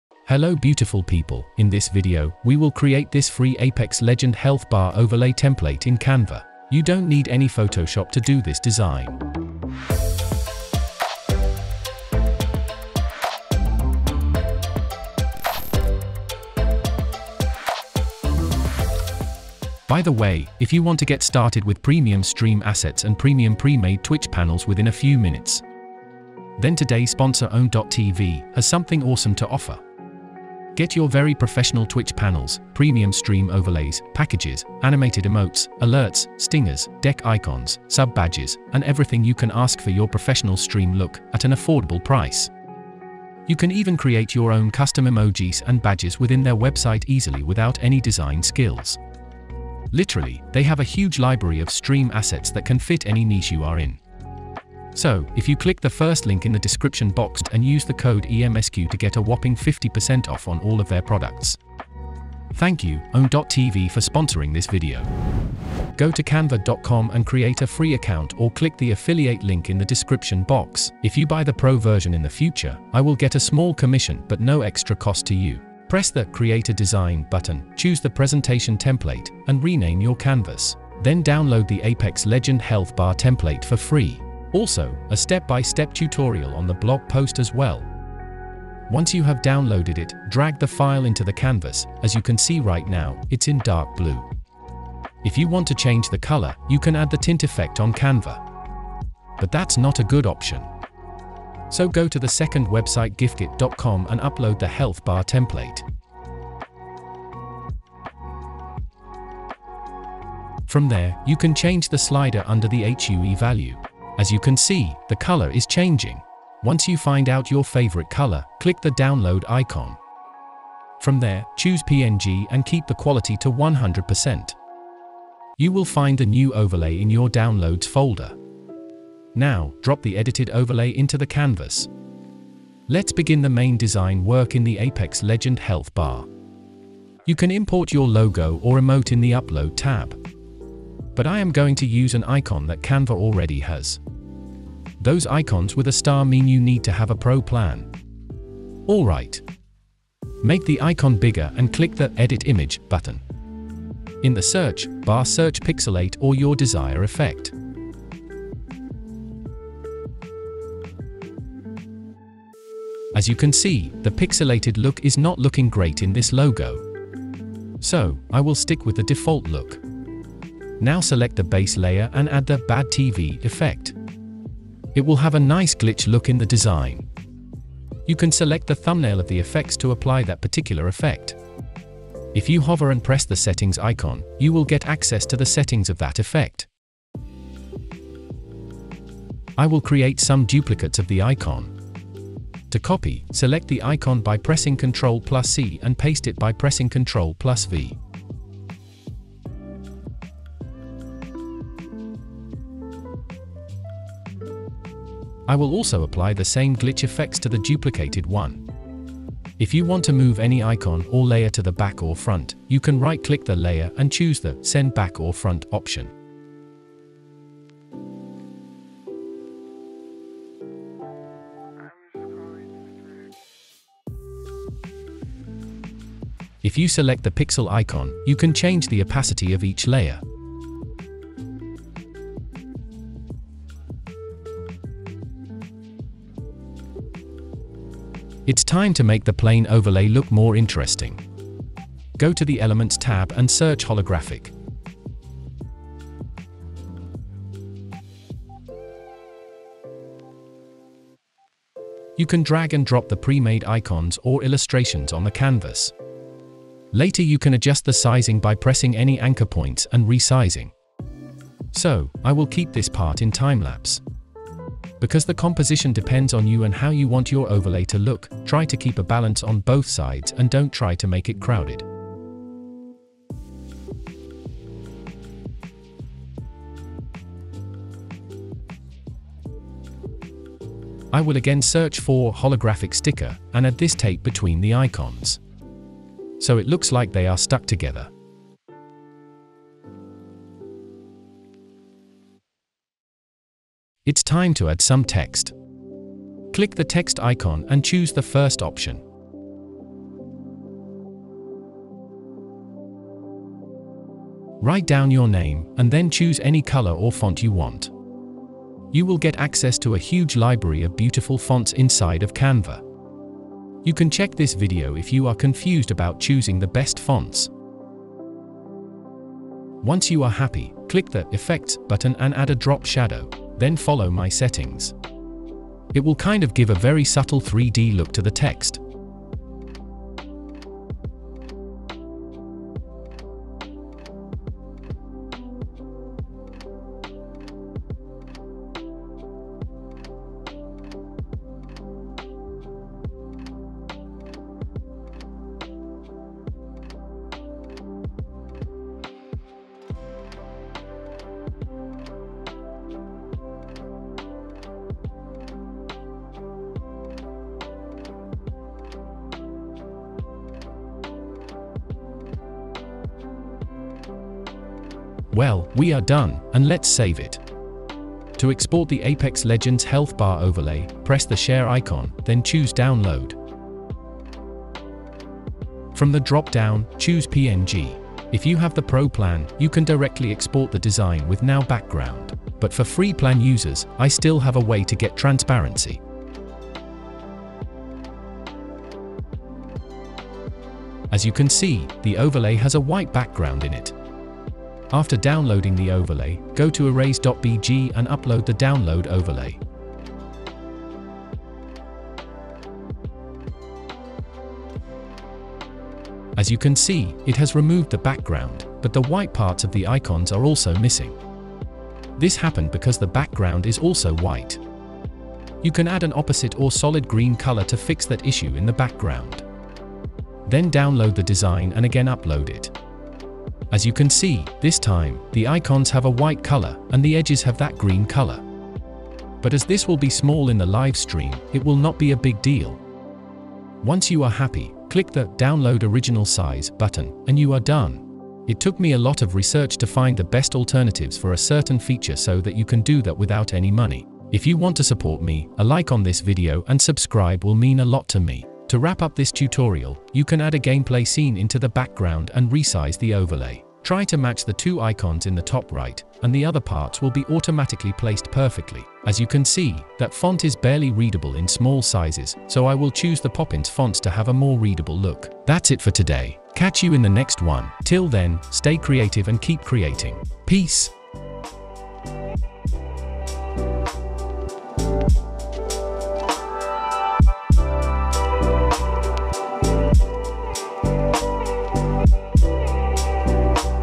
How To Make Apex Legends Health Bar Overlay - Free health bar template
In this video, I will show how to make Apex Legends Health Bar Overlay in 5 minutes on Canva.

▶Chapters:-
0:00 - Intro
0:17 - Owd3d TV AD
01:01 - Step 1: Creating the Base
01:31 - Changing the Color
02:13 - Step 2: Adding Logo + Effects
02:50 - Step 3: Adding Glitch Effect to the Base
03:11 - Step 4: Extra Elements
04:53 - Step 5: Add Text + Effects
06:07 - Export the Health Bar Overlay
07:17 - Wrap up + Outro

Discussed key points in this Video (TL;DR):
- You can learn how to create a free apex legends health bar overlay
- Free Apex legends Health bar overlay template
- Custom health bar overlay for apex legends